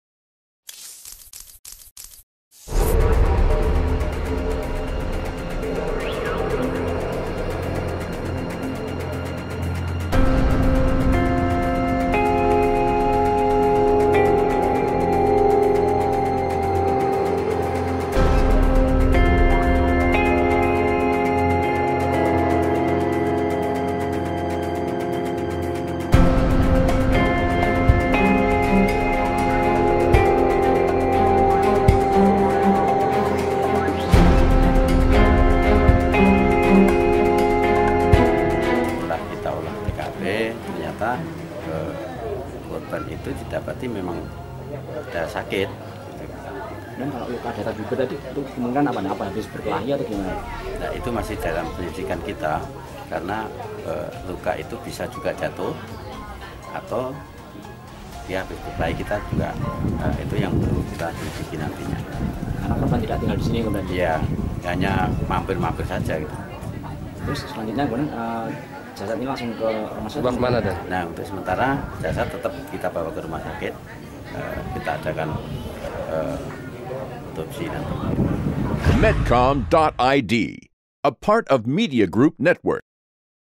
Anak Punk Ditemukan Tewas di Magetan, Diduga Dianiaya
Seoarang anak punk yang penuh tato ditemukan tewas saat berada di rumah saudaranya di Magetan, Jawa Timur. Diduga korban tewas akibat dianiaya saat berada di Solo, Jawa Tengah.

Tewasnya anak punk ini baru diketahui oleh saudaranya, Hartoyo pada Jumat  (12/8/2022) sore, kemudian saudara korban melaporkan tewasnya korban pada pihak desa dan kepolisian setempat.